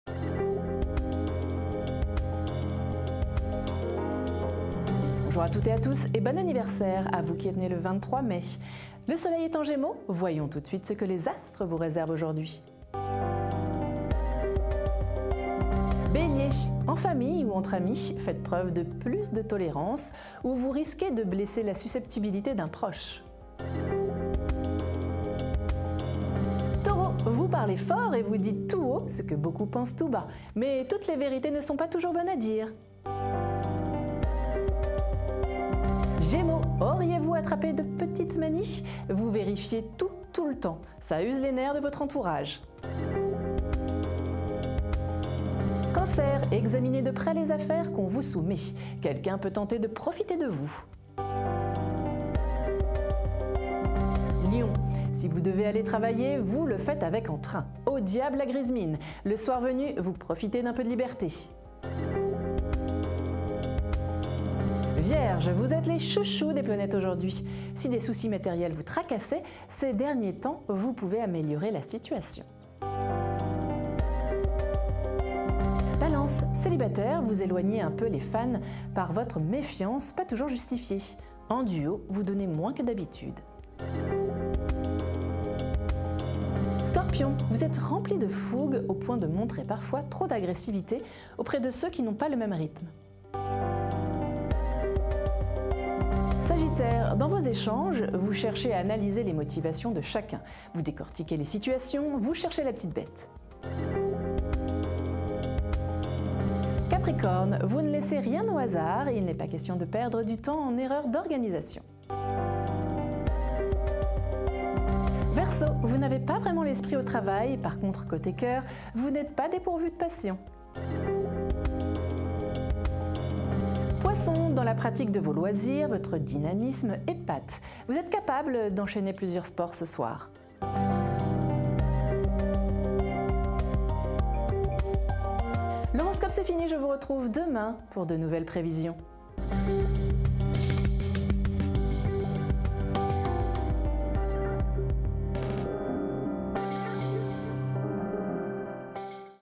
Horoscope du jour gratuit du 23 Mai 2019 par Astrocenter
Que vous réserve ce 23 Mai 2019 en amour, en famille et au travail ? Dans son horoscope du jour Ema Fontayne vous dit tout ! Quel que soit votre signe astrologique pour bien commencer votre journée retrouvez votre horoscope quotidien gratuit en vidéo.